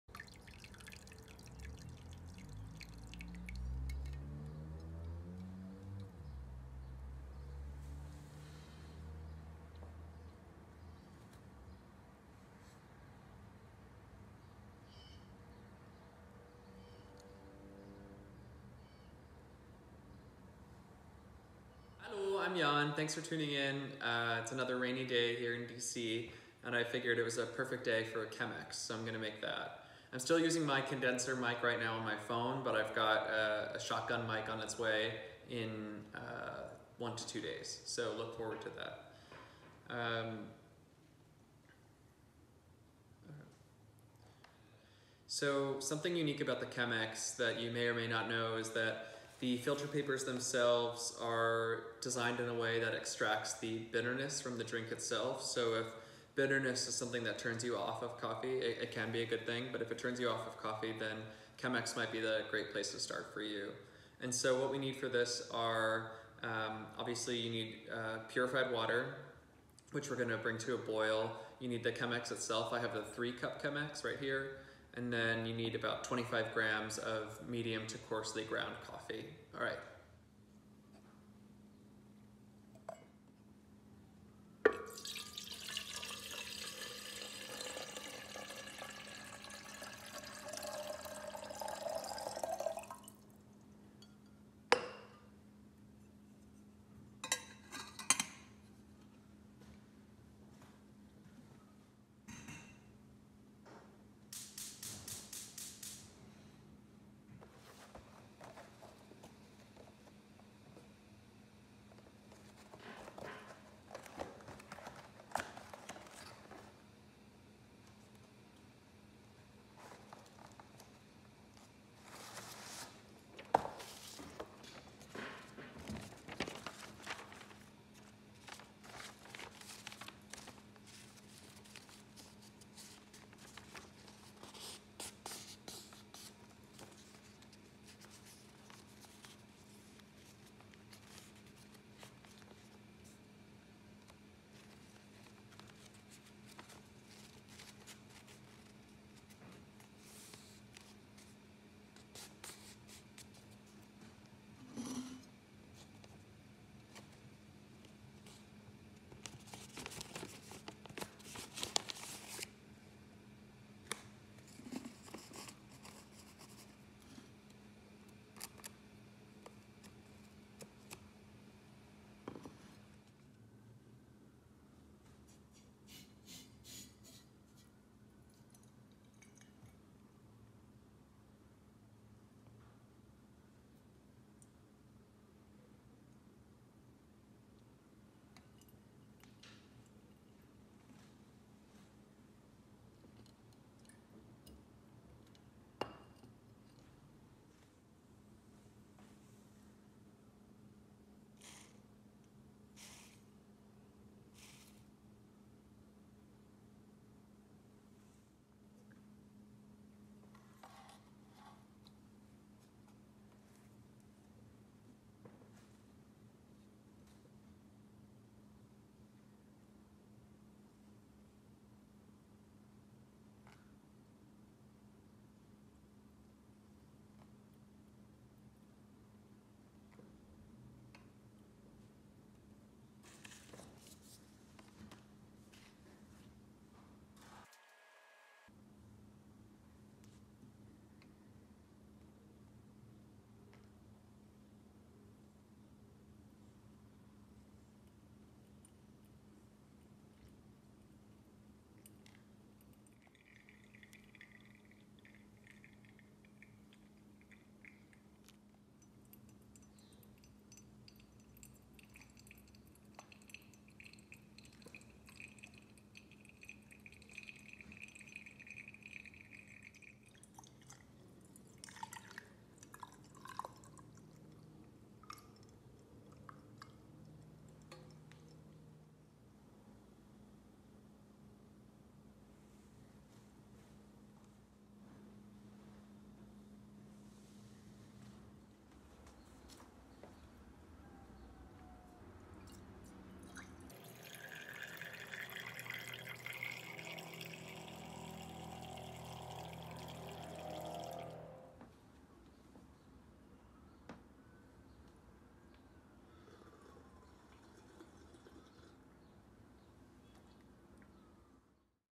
Chemex Coffee Mindfulness
Sharing with you a how-to for a Chemex 3-cup maker. This video has some ASMR but is mostly for mindfulness. The recipe I used is from Sample Coffee

Need: 25g coffee, ground medium coarse